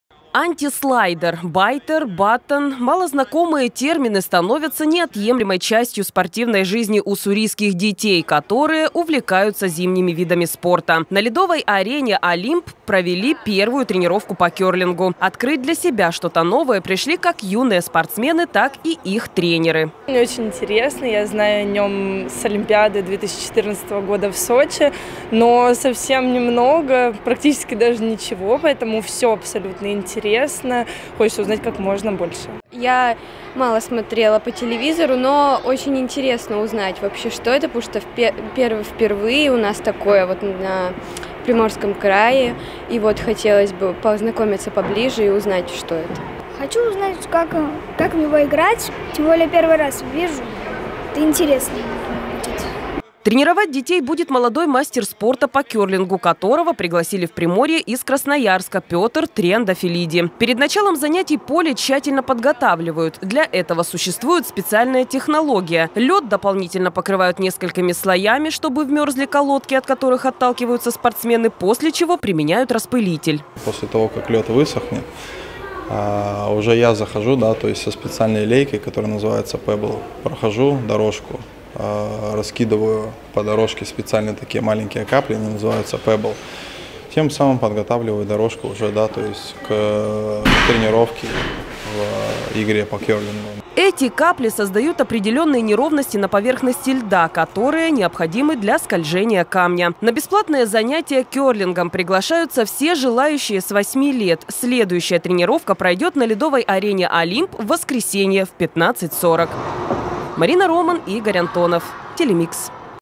Секцию кёрлинга открыли в Уссурийске
Секцию кёрлинга открыли в Уссурийске. На новой ледовой арене "Олимп" провели пробное занятие. Попробовать себя в новом для округа виде спорта смогут дети с восьми лет. Подробности узнала Марина Роман.